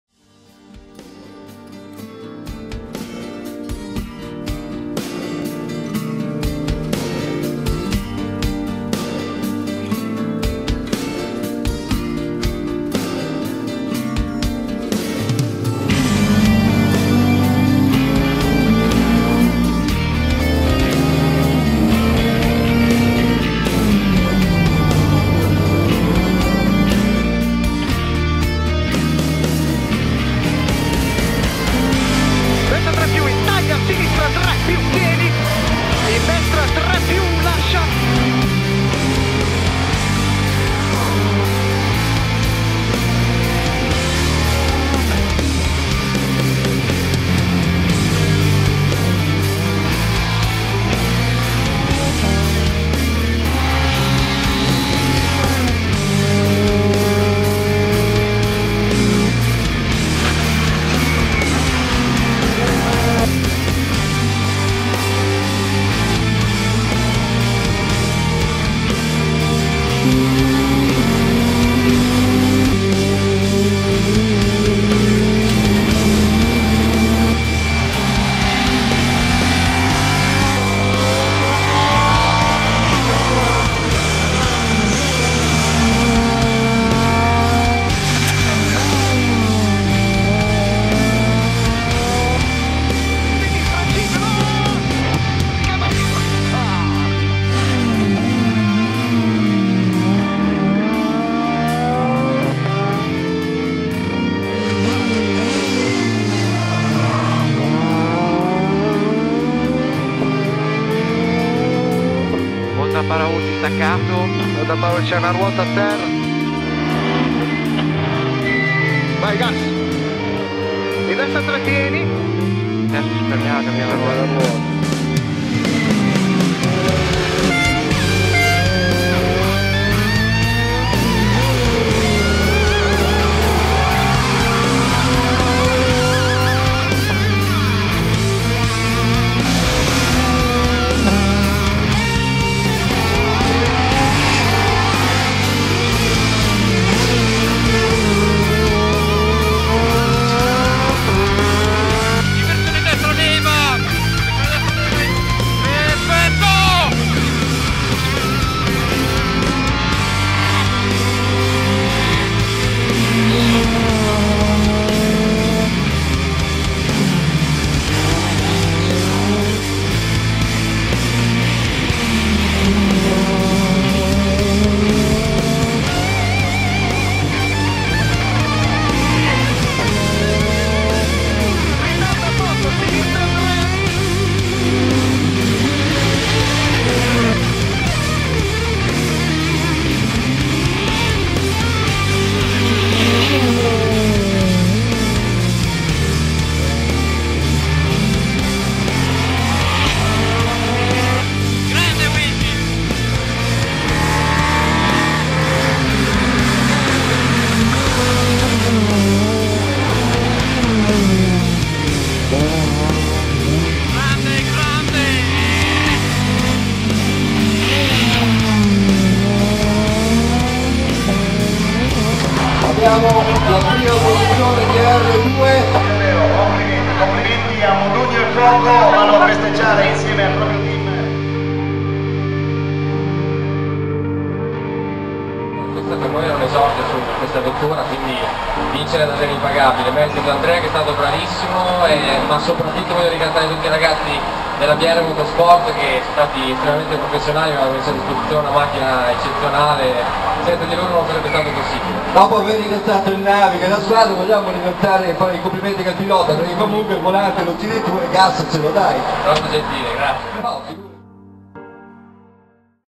2012 Rally Season
Video summary of the 2012 season that saw the crew consisting of Michele Modugno at the wheel and Andrea Fuoco at the pace notes, winners in Group R2 of the National Tarmac Rally Trophy, driving the Bierre Motorsport Citroen C2 R2 MAX.

Thanks also to Belluno Video, Video Leader, Ferrario Video and Video Dema for the kind permission of the images.